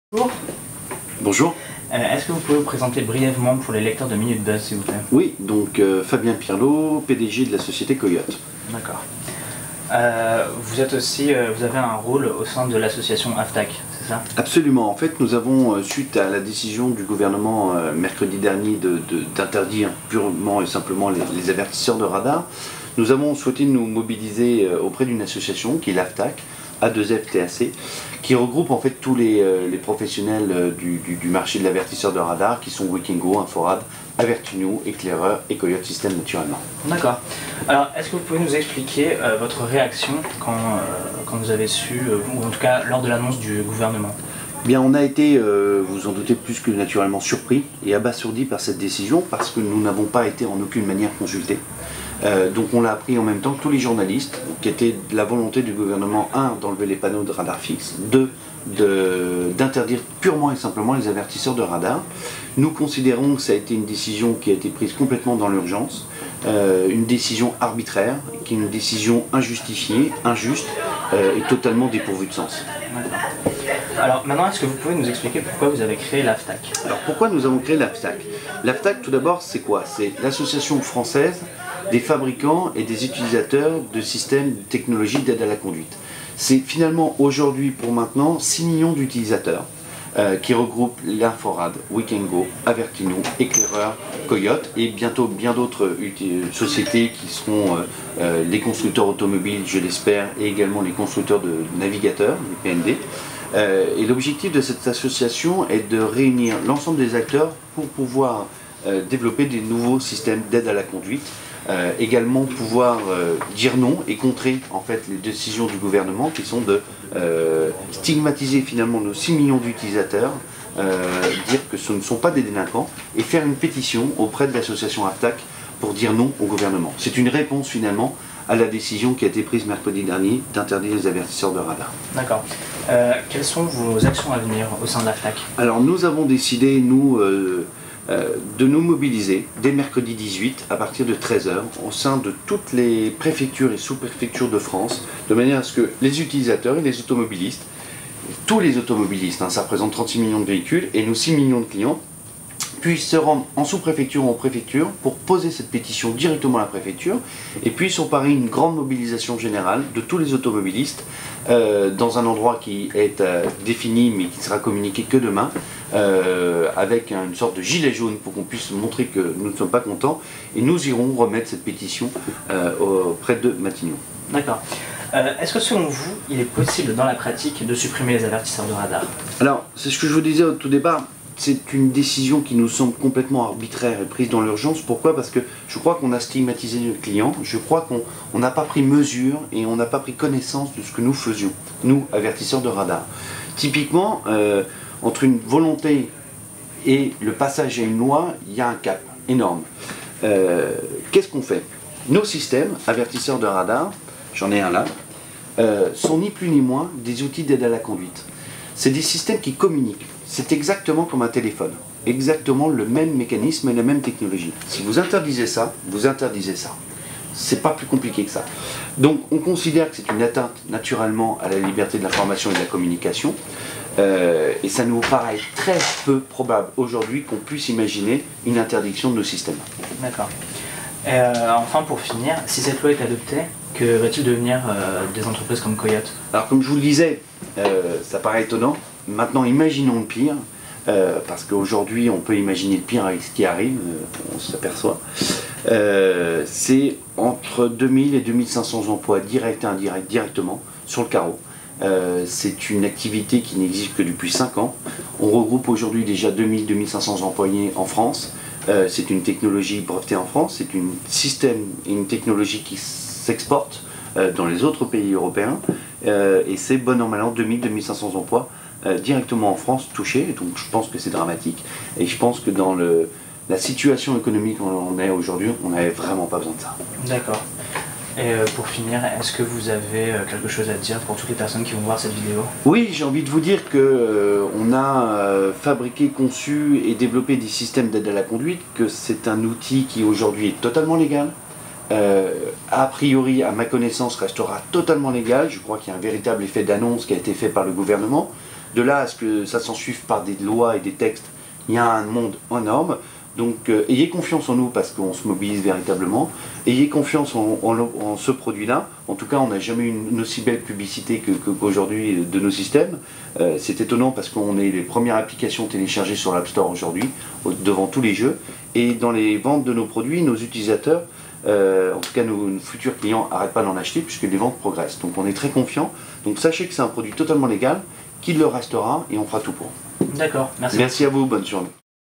Le président de Coyote et secrétaire de l'AFFTAC s'exprime
Le président de Coyote et secrétaire de l'AFFTAC s'exprime à propos des événements de ces derniers jours.